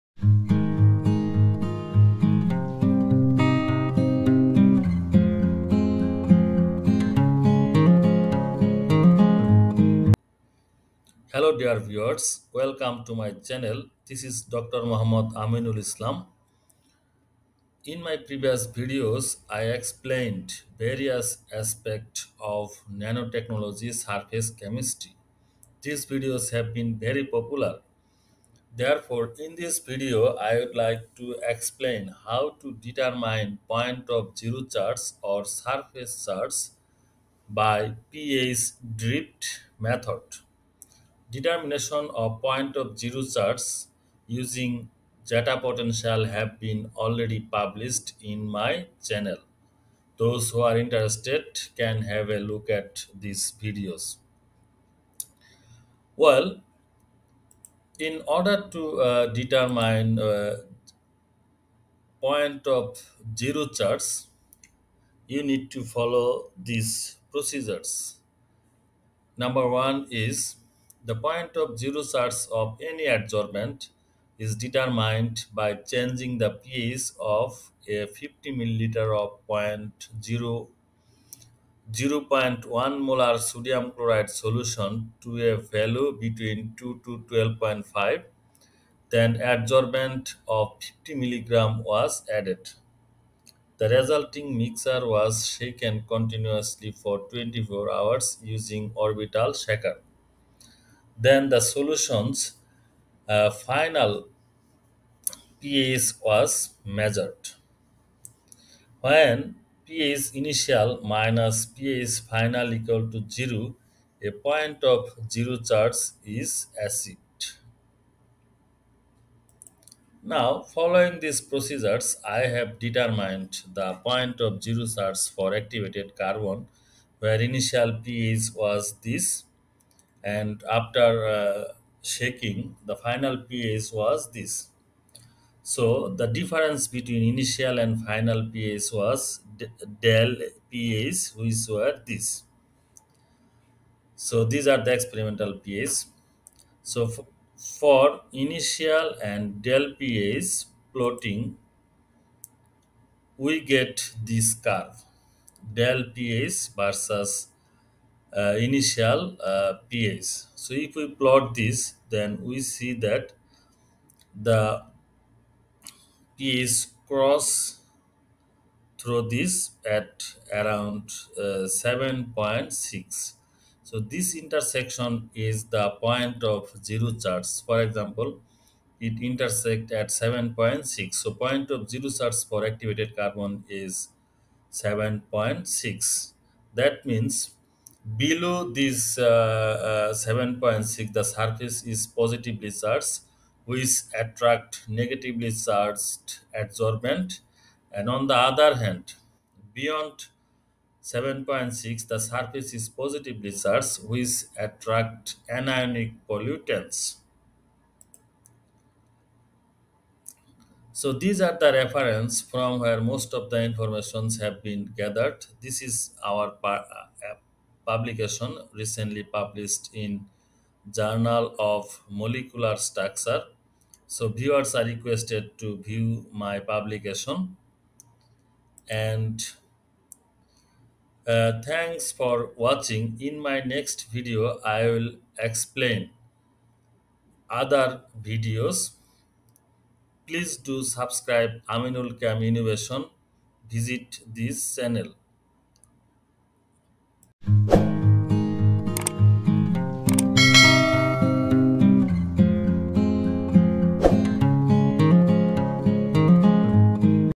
Determination of point of zero charge by pH drift method
This lesson is for all who are studying adsorption or surface chemistry and analyzing their experimental data for writing a thesis presentation or publishing. This video explains How to determine of point of zero charge by pH drift method easily.

 / aminulchemi.  .